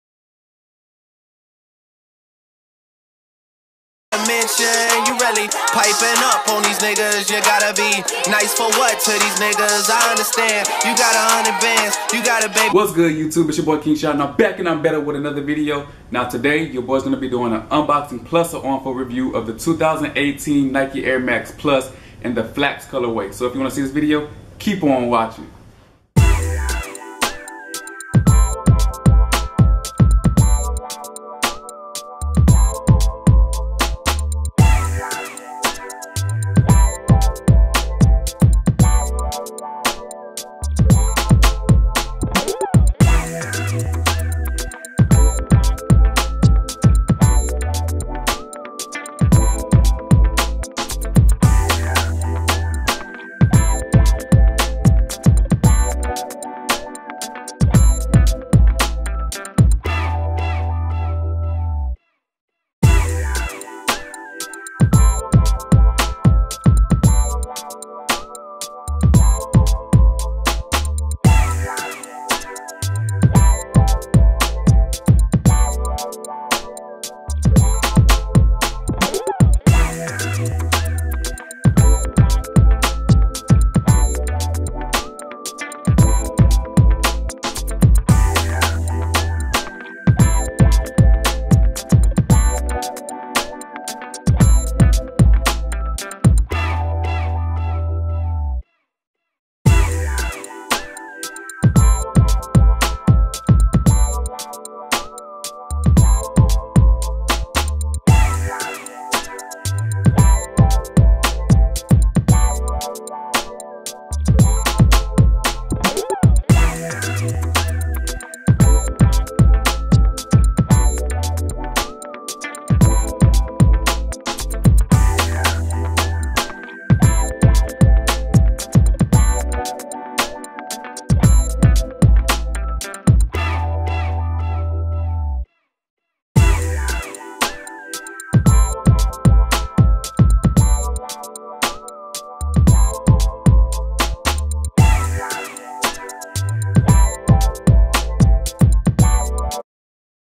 Flax Nike Air Max Plus Unoboxing & On Foot Review!
Here is my Unboxing of the Flax 2018 Nike Air Max Plus TN.